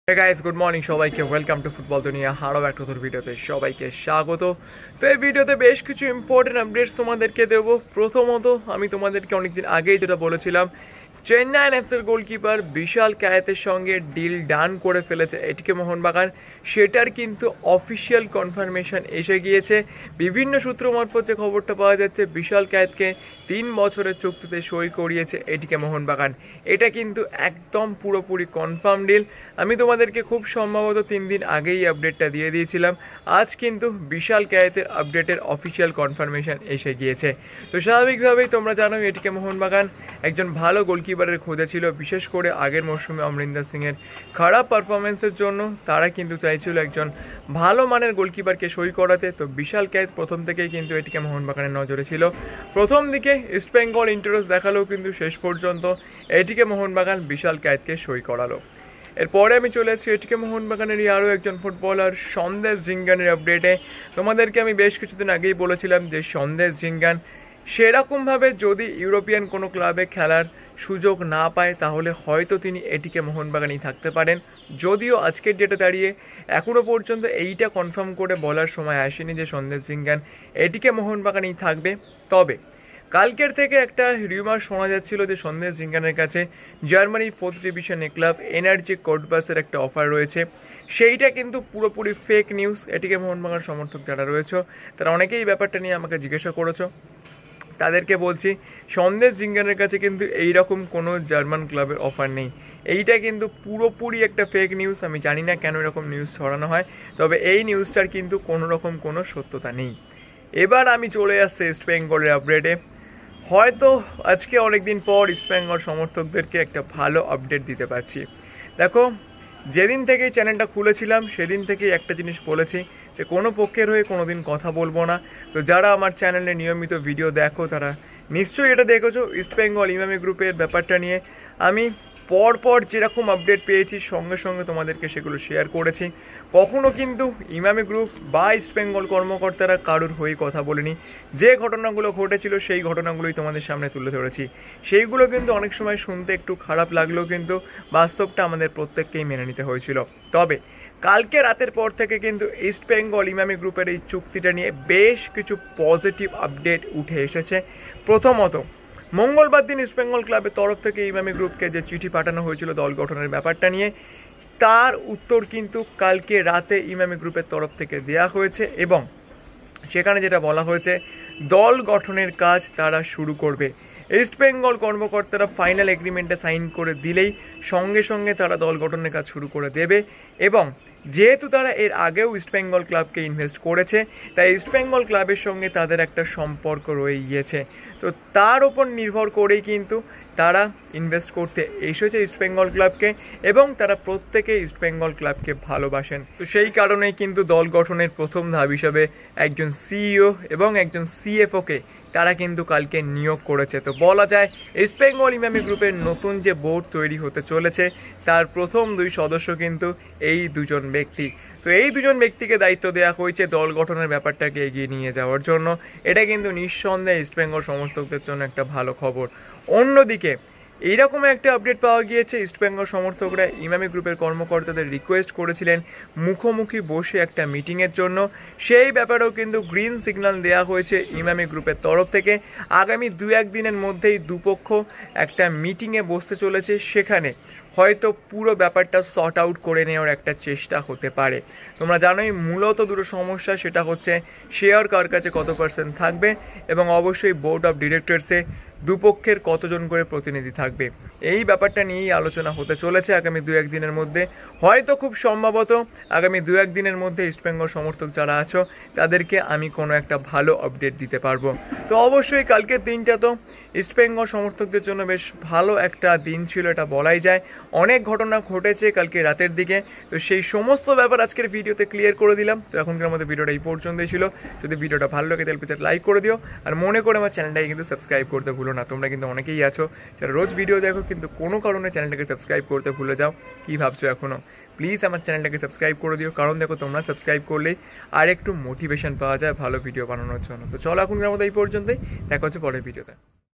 সমস্যা মেটাতে বৈঠকে বসতে চলেছে EastBengal Emami🤔ATK MohunBagan সই করলেন Vishal Kaith✅Sandesh Jhingan
PLEASE FOLLOW MEbu